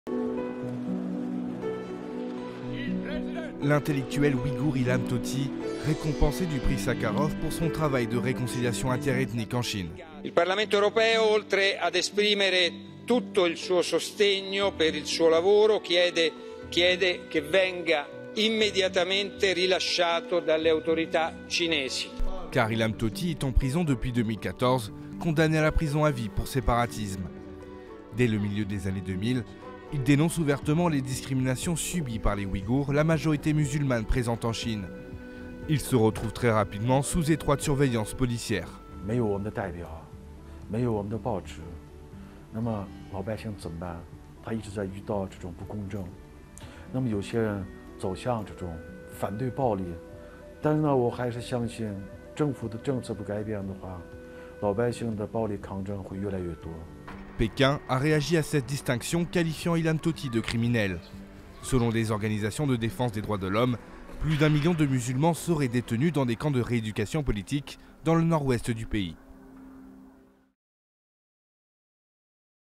Le résistant ouïghour Ilham Tohti est couronné du Prix Sakharov
Le Parlement européen a décerné jeudi le prix Sakharov des droits de l'homme à l'intellectuel ouïghour Ilham Tohti, condamné à la prison à vie en Chine pour "séparatisme", au risque de hérisser Pékin, a-t-on appris auprès des groupes politiques.